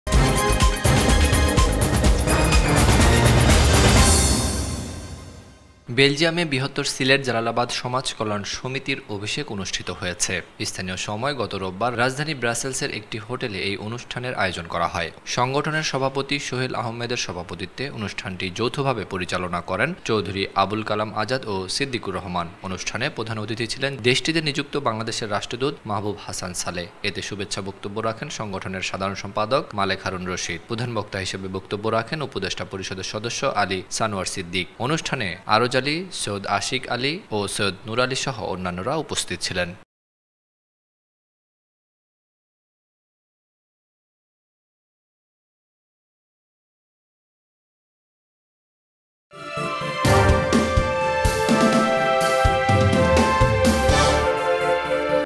বেলজিয়ামে বৃহত্তর সিলেট জালালাবাদ সমাজ কল্যাণ সমিতির অভিষেক অনুষ্ঠিত হয়েছে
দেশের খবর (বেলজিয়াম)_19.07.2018